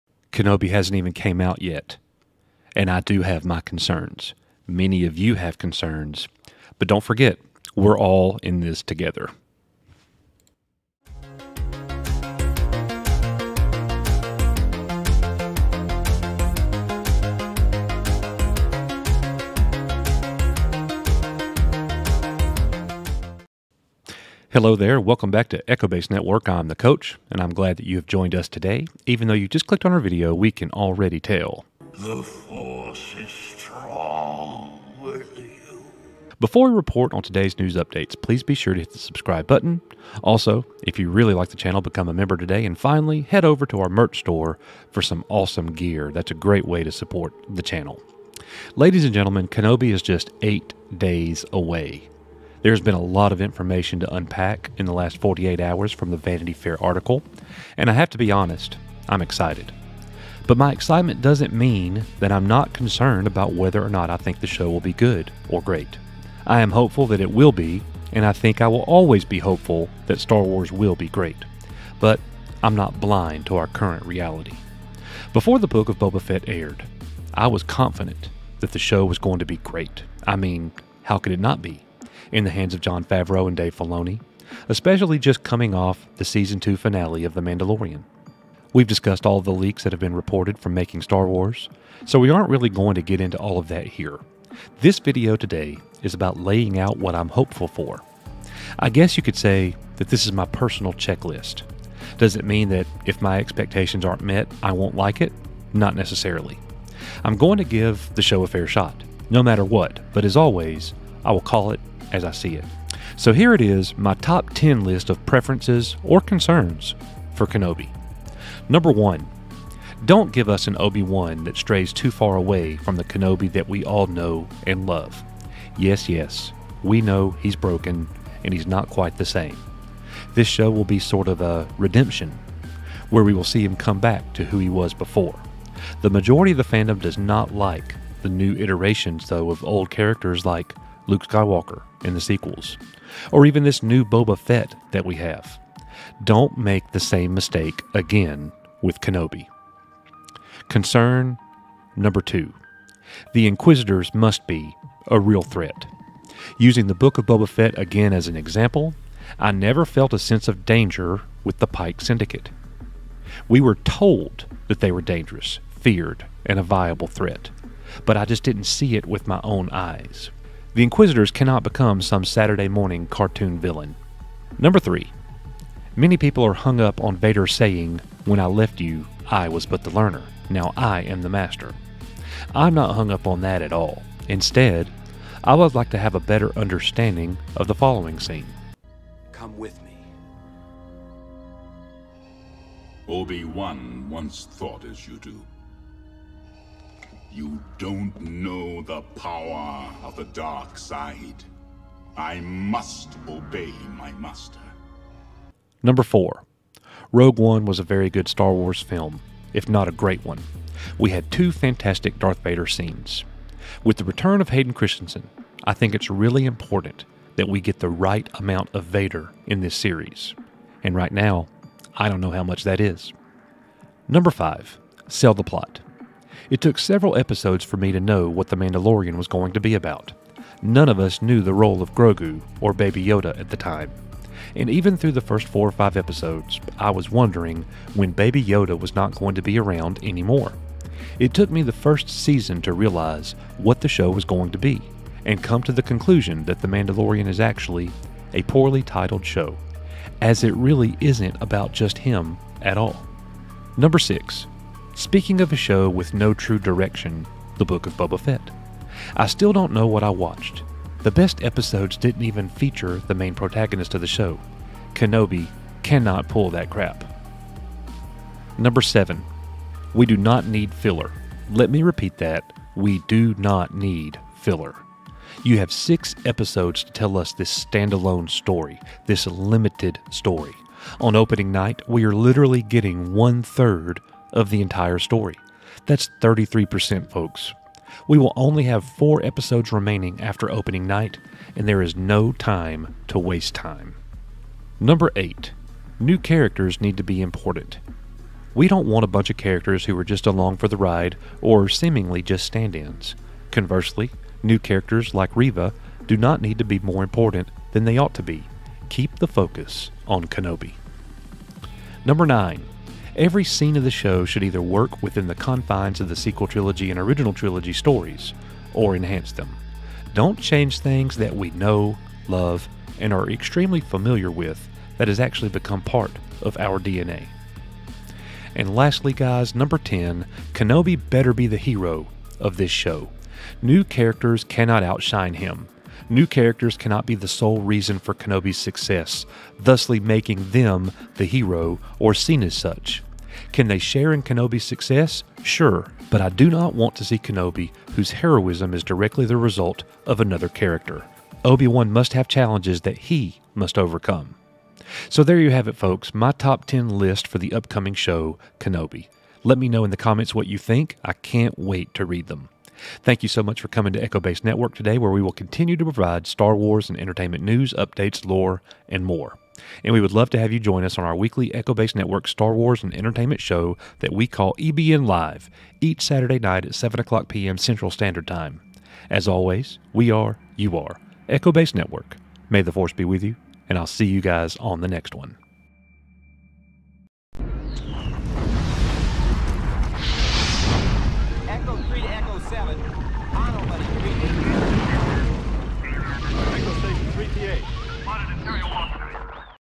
Top 10 Concerns for Kenobi! Star Wars News Update
Many fans are concerned with the upcoming Star Wars Disney+ limited series, Obi-Wan Kenobi.  In today's episode, I run us through my top 10 reasons for concerns!